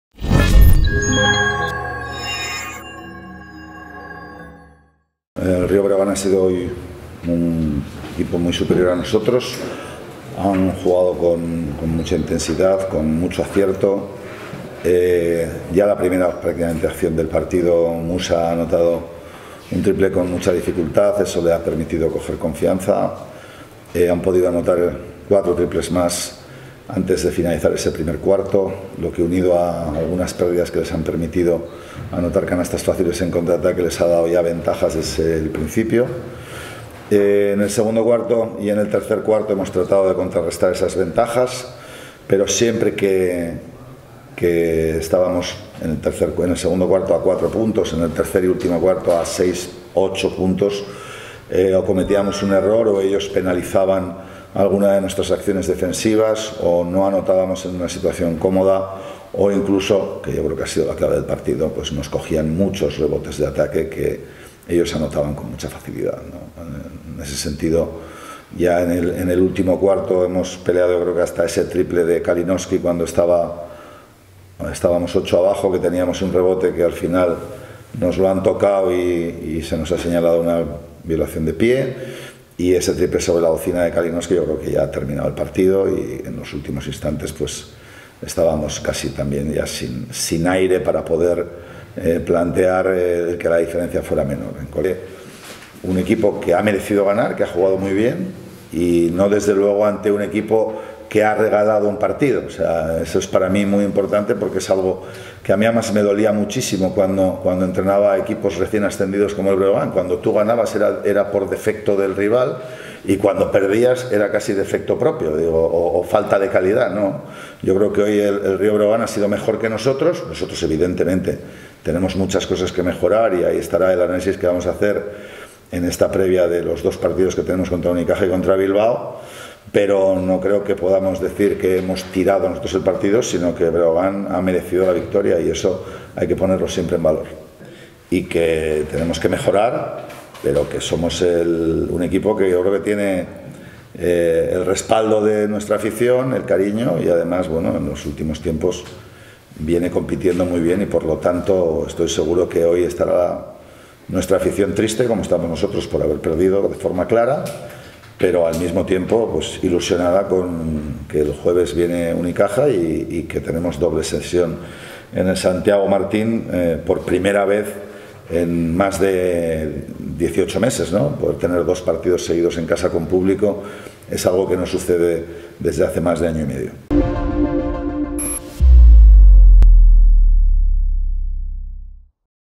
Pospartido del Rio Breogán-Lenovo Tenerife (92-73)
Declaraciones de Txus Vidorreta posteriores al Rio Breogán-Lenovo Tenerife (92-73) | Liga Endesa 2021-22

🎙️  "El Breogán  ha sido muy superior a nosotros. Han jugado con mucha intensidad y acierto"

🎙️ "Ellos han merecido ganar" [...] "Nosotros tenemos muchas cosas que mejorar"